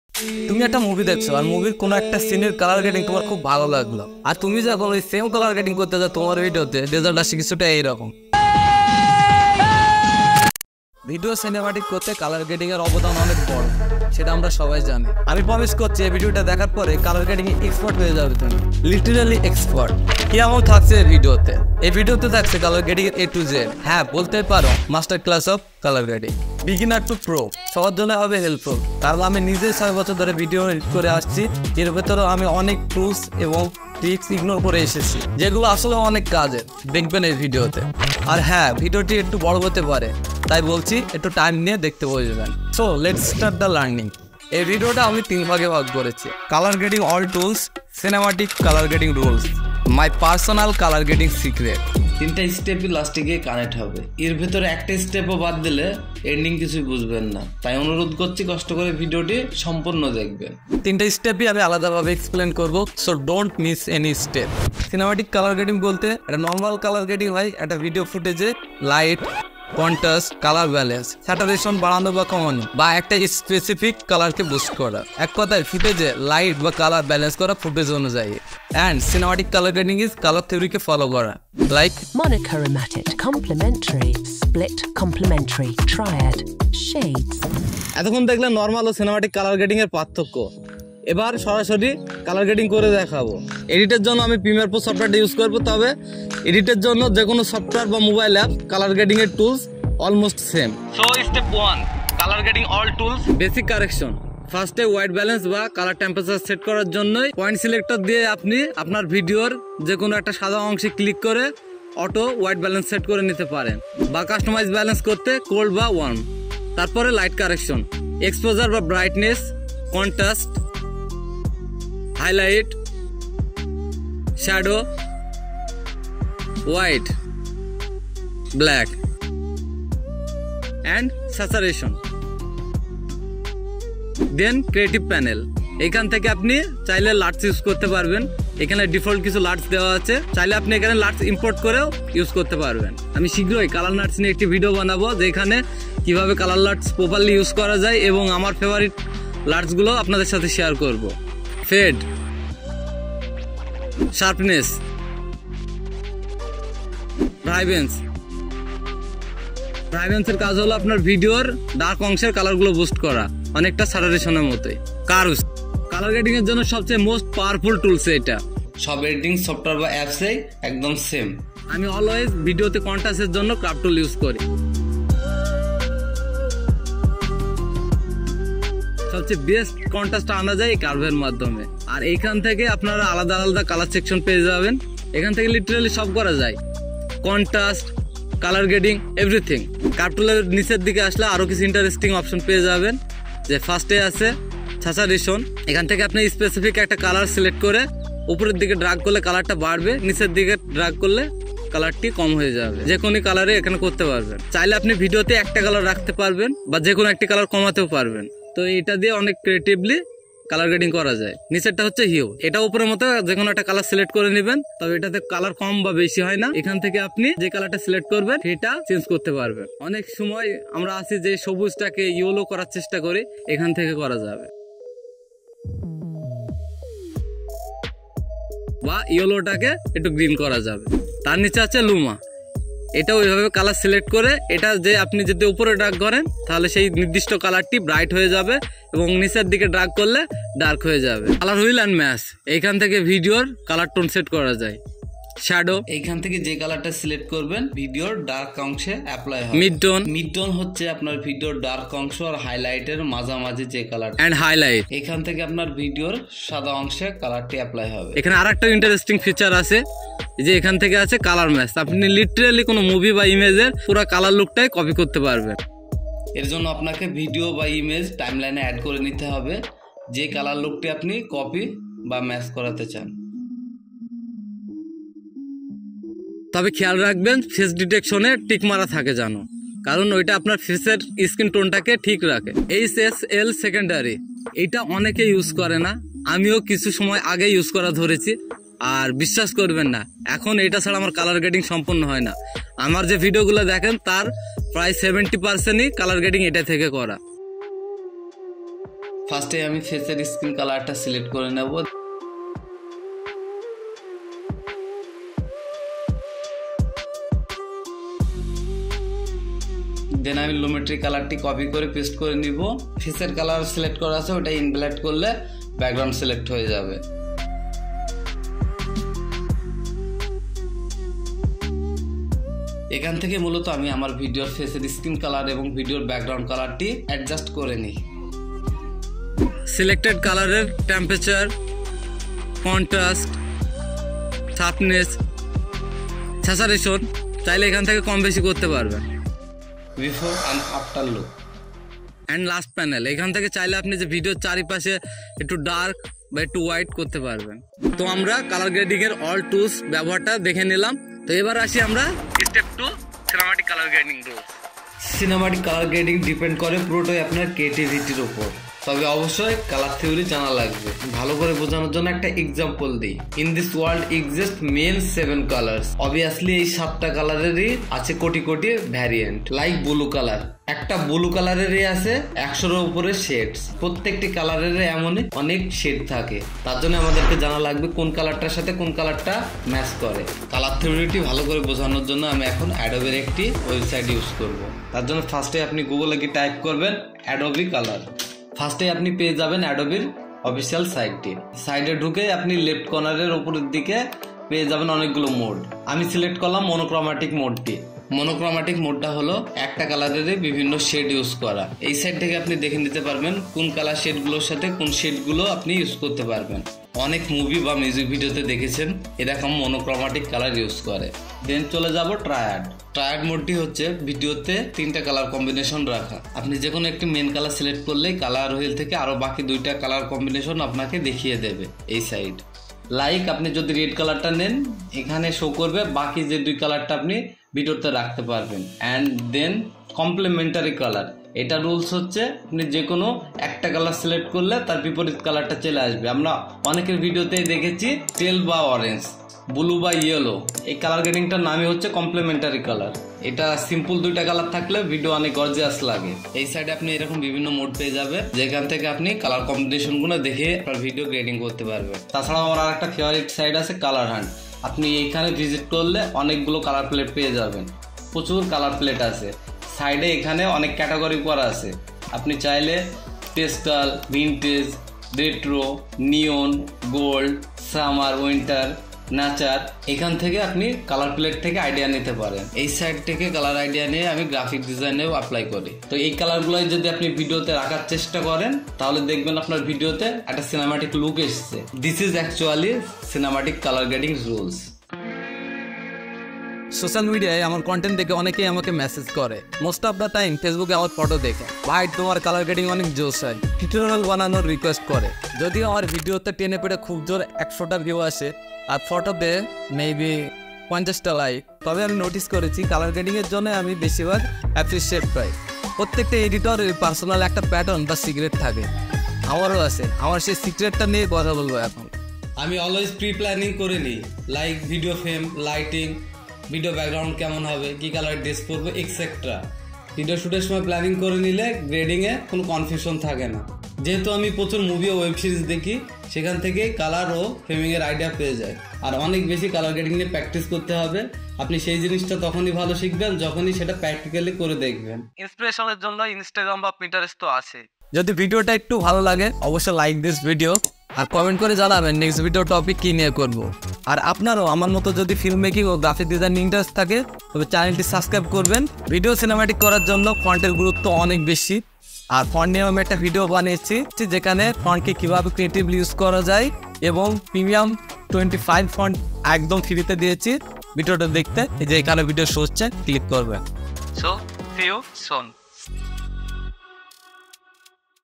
Color Grading in Premiere Pro | Masterclass A to Z
➤Cinematic Color Secret | Color Grading Masterclass

Music from Uppbeat (free for Creators!)

Time Chapter -
00:00 - Intro
01:04 - Normal VS Cinematic
01:28 - Step1 - Color Grading All Tools
02:12 - Basic Correction
02:45 - Creative Panel
04:01 - Curve
04:43 - Color Wheel & Match
05:48 - HSL Secondary
05:42 - Vignette
05:47 - Step2 - Cinematic Color Theory
08:05 - Step3 - My Secret
08:57 - Outro